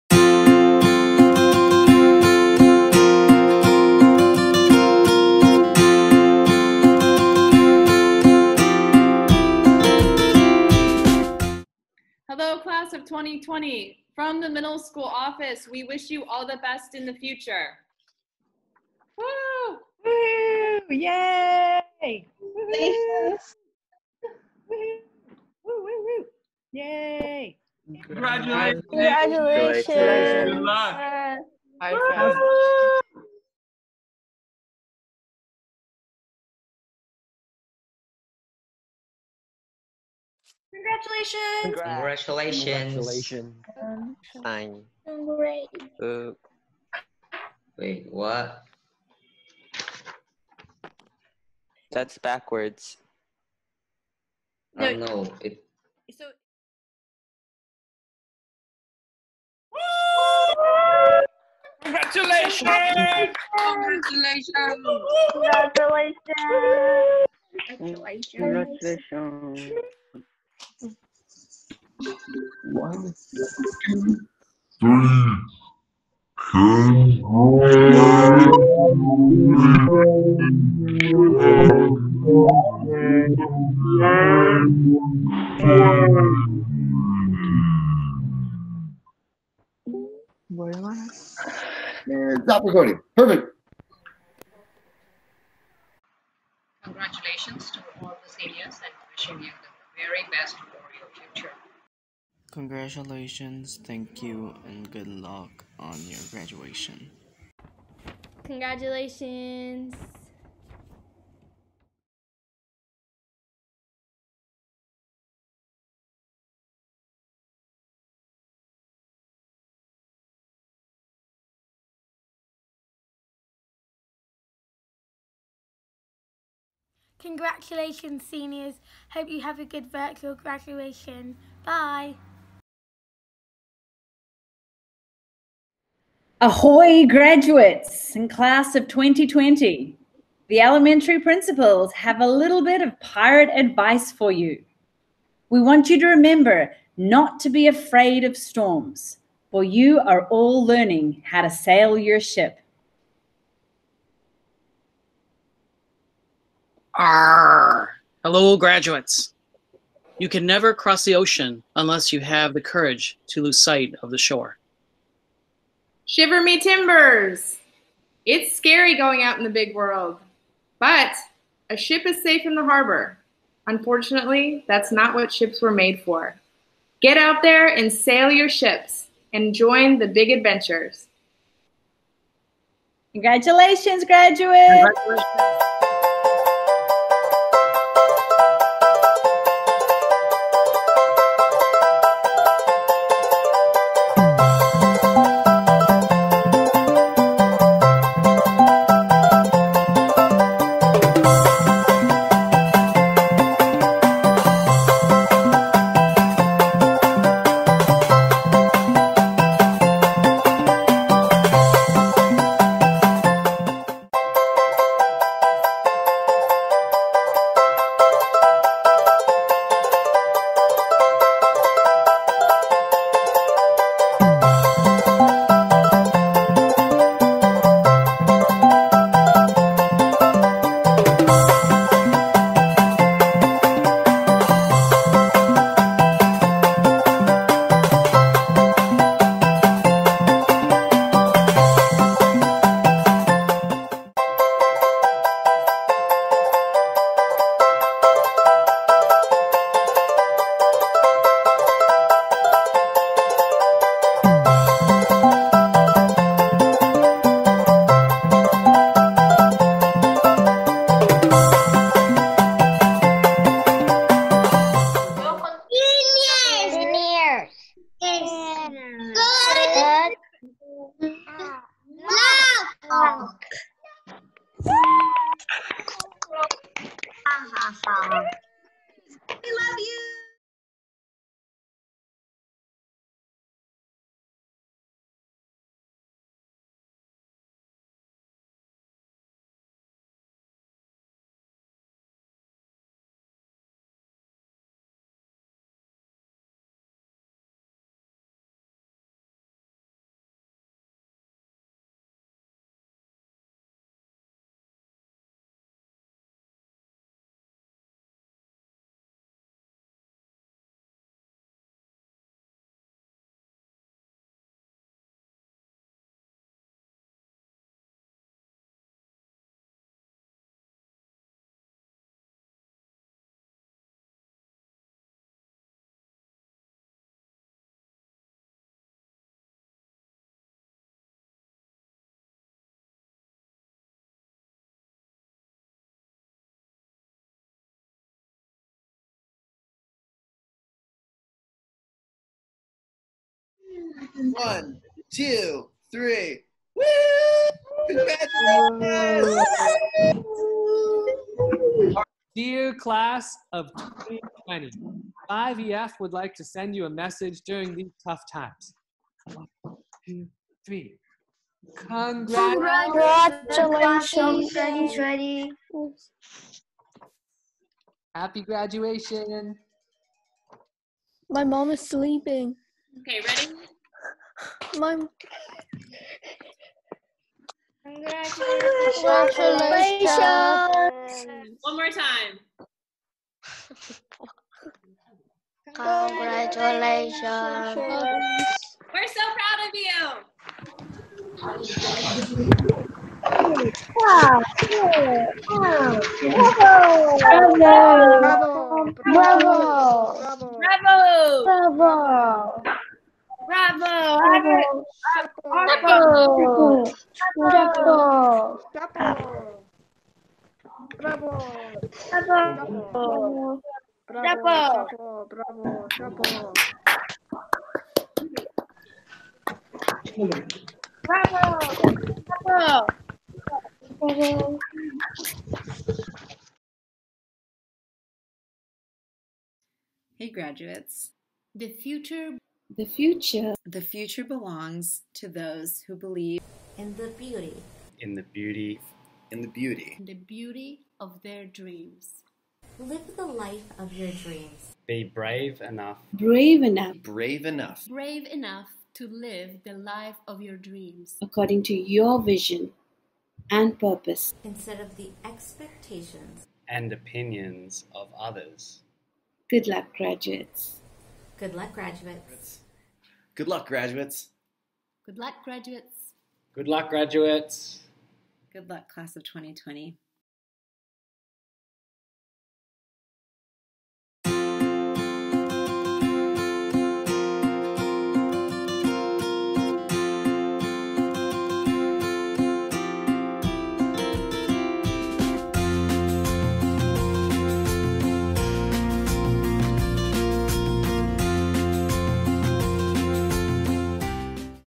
Virtual Grad Walk 2020 | AMERICAN INTERNATIONAL SCHOOL CHENNAI
Our Elementary and Middle Schoolers join in a virtual graduation walk in honor of our pathbreaking Class of 2020!

Congratulations, Seniors! We are so proud of you!